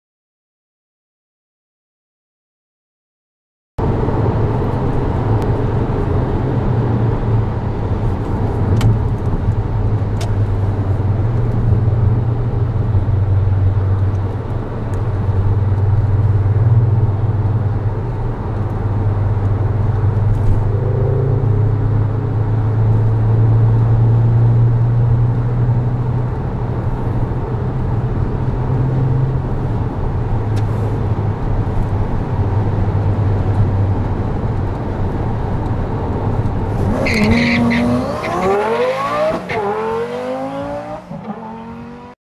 Spotter Video: Shelby GT500 in Munich - Road Test TV ®
Crew of Road Test TV spots a Shelby GT500 in Munich, Germany.